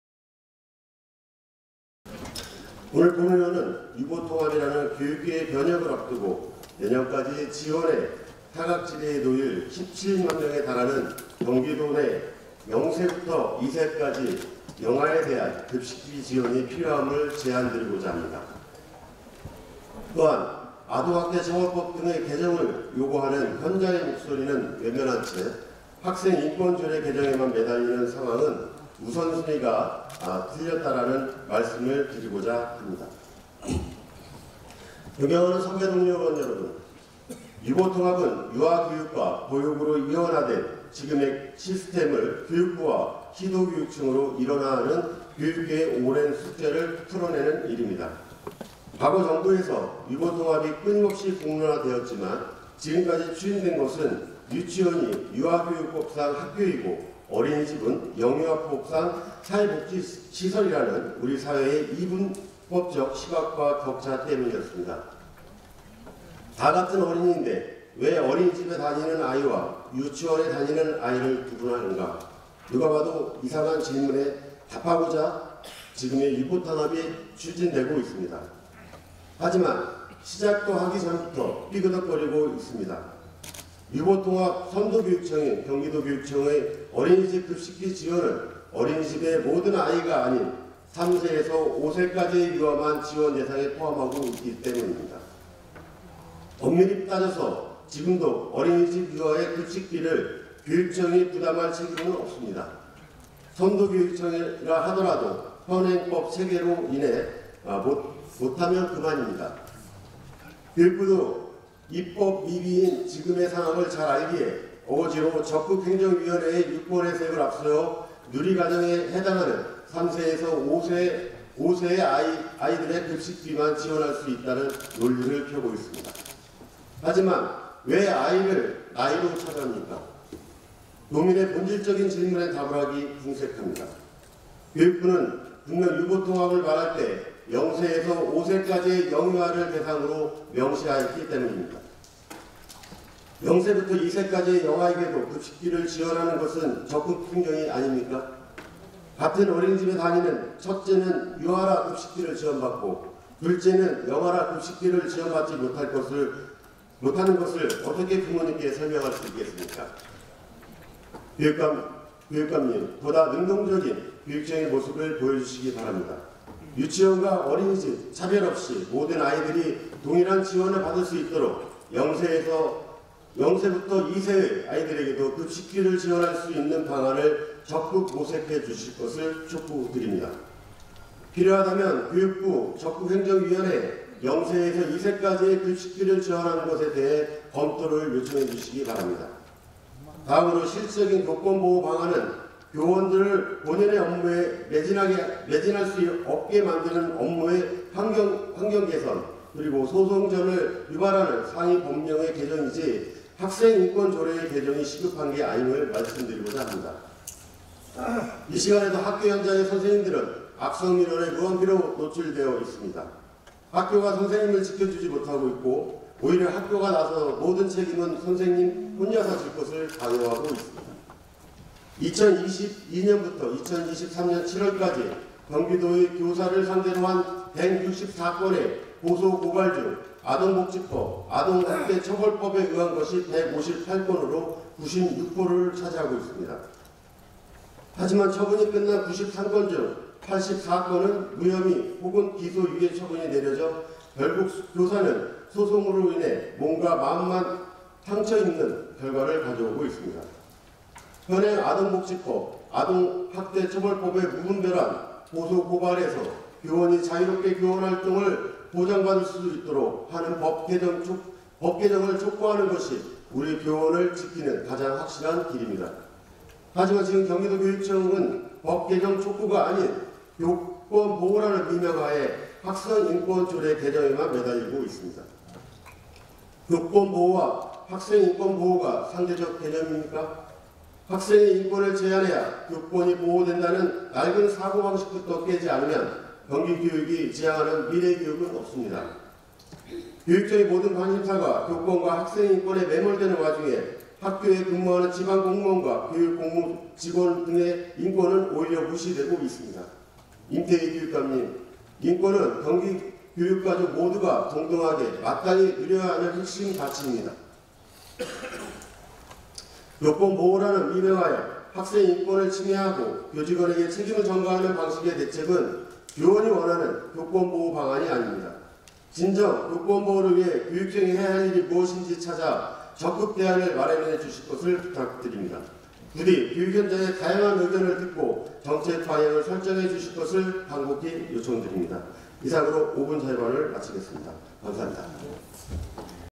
교육청에 ‘유보통합 관련 영아 급식비 지원’ 및 ‘실질적인 교권 보호를 위한 대책 이행’ 촉구
[양해용 기자]=경기도의회 교육행정위원회 안광률 부위원장(더불어민주당, 시흥1)은 6일(수) 제371회 임시회 제2차 본회의에서 5분 자유발언을 통해, 유보통합 추진 과정에서 17만명에 달하는 경기도 내 0세부터 2세까지의 영아에 대해도 경기도교육청의 빠짐없는 급식비 지원이 필요함을 강조하였다. 또한, 실질적인 교권 보호를 위해 경기도교육감이 학생인권조례 개정이 아닌 교육환경 개선과 「아동학대처벌법」 등의 개정 노력에 먼저 나설 것을 촉구하였다.